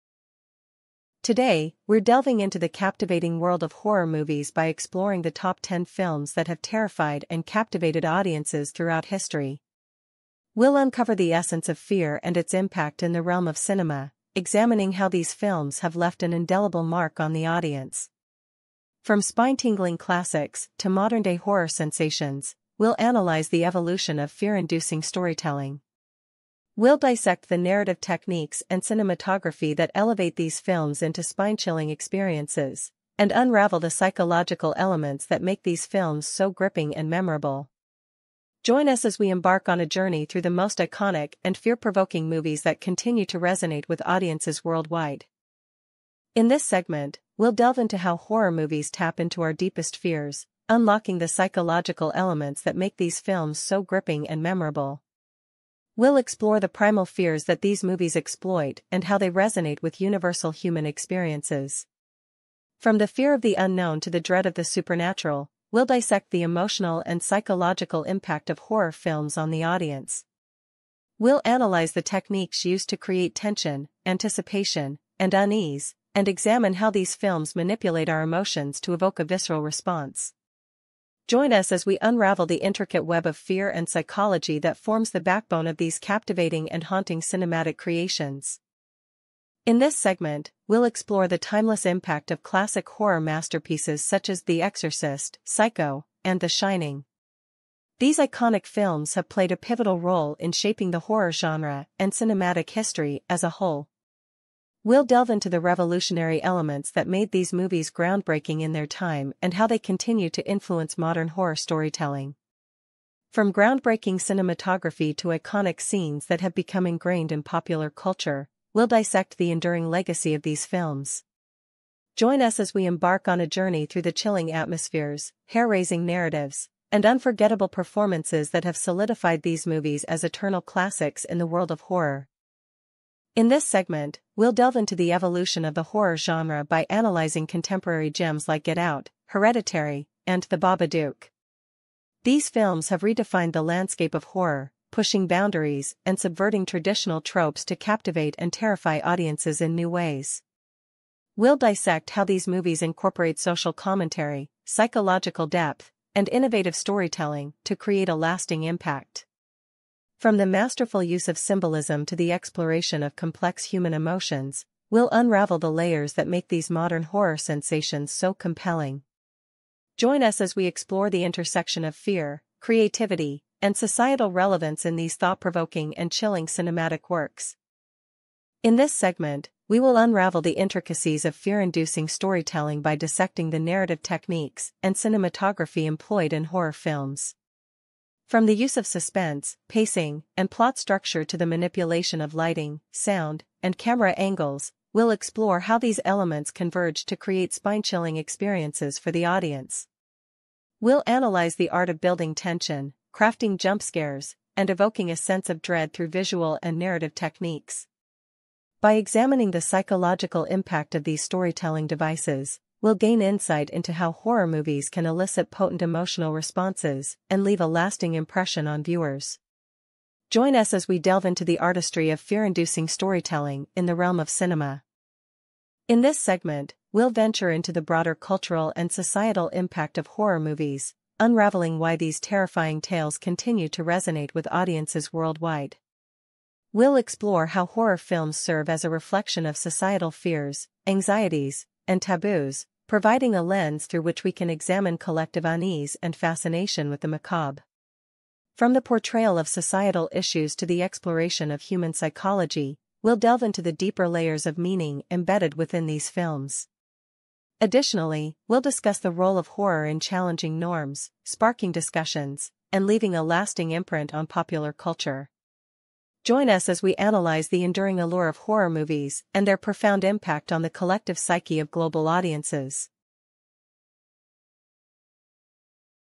Embracing Fear: Exploring The Top 10 Horror Movies Of All Time
In this spine-chilling exploration, we dive into the captivating world of horror movies, dissecting the top 10 films that have terrified and captivated audiences for generations. From timeless classics like 'The Exorcist' and 'Psycho' to modern sensations such as 'Get Out' and 'Hereditary', we unravel the psychological elements and narrative techniques that make these films so hauntingly memorable. Join us as we navigate through the evolution of fear-inducing storytelling, delving into the artistry that elevates these movies into unforgettable, spine-chilling experiences.

Beyond the scares, we examine the broader cultural and societal impact of horror movies, shedding light on why these terrifying tales continue to resonate with audiences worldwide. From societal fears and anxieties to the exploration of human psychology, these films offer a reflection of our collective unease and fascination with the macabre. We unveil the profound impact of horror in challenging norms, sparking discussions, and leaving a lasting imprint on popular culture, giving viewers a deeper appreciation for the genre's complexity and cultural resonance.

Whether you're a dedicated horror aficionado or a curious observer, this video promises to provide a thought-provoking journey through the most iconic and fear-inducing movies in cinematic history. Embrace the fear, delve into the darkness, and unravel the enthralling complexities that make horror movies an enduring and captivating genre, shaping the collective psyche of global audiences.

horror movies, top 10, fear-inducing, classic, modern, psychological elements, cinematic history, storytelling, societal impact, cultural resonance

Today, we're delving into the captivating world of horror movies by exploring the top 10 films that have terrified and captivated audiences throughout history. We'll uncover the essence of fear and its impact in the realm of cinema, examining how these films have left an indelible mark on the audience. From spine-tingling classics to modern-day horror sensations, we'll analyze the evolution of fear-inducing storytelling. We'll dissect the narrative techniques and cinematography that elevate these films into spine-chilling experiences, and unravel the psychological elements that make these films so gripping and memorable. Join us as we embark on a journey through the most iconic and fear-provoking movies that continue to resonate with audiences worldwide.
In this segment, we'll delve into how horror movies tap into our deepest fears, unlocking the psychological elements that make these films so gripping and memorable. We'll explore the primal fears that these movies exploit and how they resonate with universal human experiences. From the fear of the unknown to the dread of the supernatural, we'll dissect the emotional and psychological impact of horror films on the audience. We'll analyze the techniques used to create tension, anticipation, and unease, and examine how these films manipulate our emotions to evoke a visceral response. Join us as we unravel the intricate web of fear and psychology that forms the backbone of these captivating and haunting cinematic creations.
In this segment, we'll explore the timeless impact of classic horror masterpieces such as 'The Exorcist', 'Psycho', and 'The Shining'. These iconic films have played a pivotal role in shaping the horror genre and cinematic history as a whole. We'll delve into the revolutionary elements that made these movies groundbreaking in their time and how they continue to influence modern horror storytelling. From groundbreaking cinematography to iconic scenes that have become ingrained in popular culture, we'll dissect the enduring legacy of these films. Join us as we embark on a journey through the chilling atmospheres, hair-raising narratives, and unforgettable performances that have solidified these movies as eternal classics in the world of horror.
In this segment, we'll delve into the evolution of the horror genre by analyzing contemporary gems like 'Get Out', 'Hereditary', and 'The Babadook'. These films have redefined the landscape of horror, pushing boundaries and subverting traditional tropes to captivate and terrify audiences in new ways. We'll dissect how these movies incorporate social commentary, psychological depth, and innovative storytelling to create a lasting impact. From the masterful use of symbolism to the exploration of complex human emotions, we'll unravel the layers that make these modern horror sensations so compelling. Join us as we explore the intersection of fear, creativity, and societal relevance in these thought-provoking and chilling cinematic works.
In this segment, we will unravel the intricacies of fear-inducing storytelling by dissecting the narrative techniques and cinematography employed in horror films. From the use of suspense, pacing, and plot structure to the manipulation of lighting, sound, a